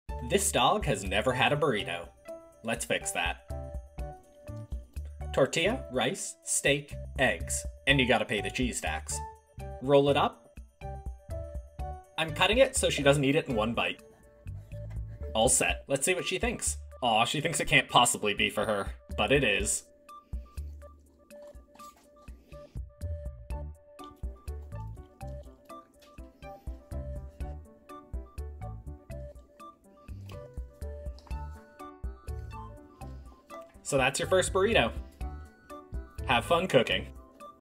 Dog Gets Her First Burrito! It Was Delicious!
It is a very special day for this dog. I'm making her a burrito out of foods that are safe for dogs, and that she loves!
She is pretty adorable.
Delicious, quick, and super cute!